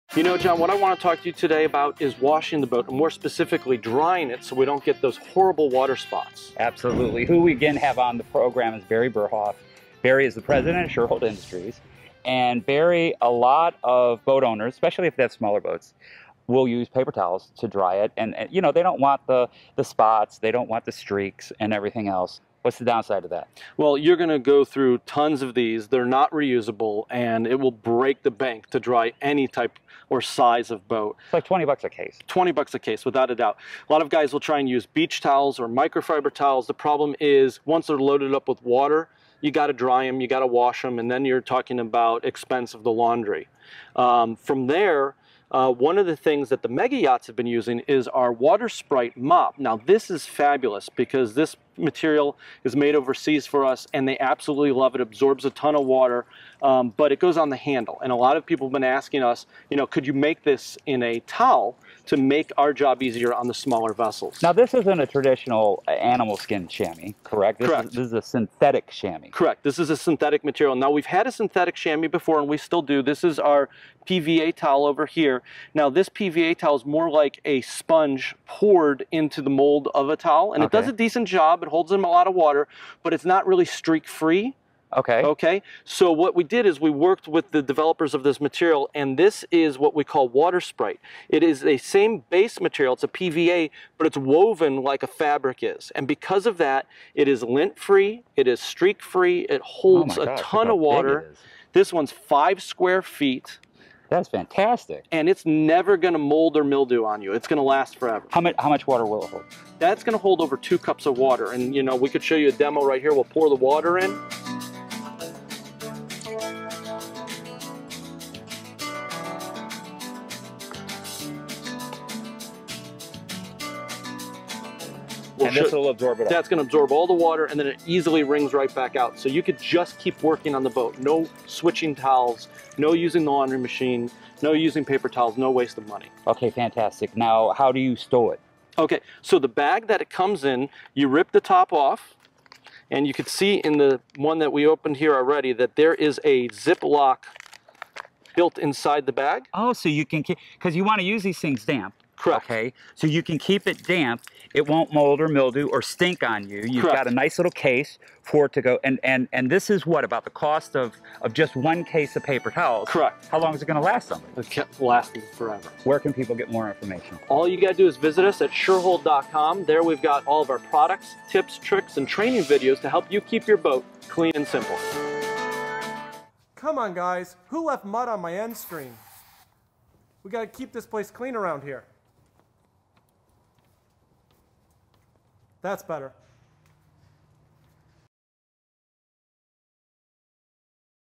Take a look a Shurhold's new Deluxe Water Sprite Hand Towel!
Today, Ship Shape TV's John Greviskis stopped by to see Shurhold's Barry Berhoff and discuss the brand new Deluxe Water Sprite Hand Towel! The most advanced hand chamois ever made, it's designed using the same woven PVA material that made our Deluxe Water Sprite Mops famous and an industry standard worldwide. Incredibly resistant to mold and mildew, you can rely on this super absorbent chamois for years to come, for roughly the same price as a case of paper towels.

Want to connect? You can follow Shurhold on all the major social platforms.

Head to Shurhold.com for more great boat detailing tips!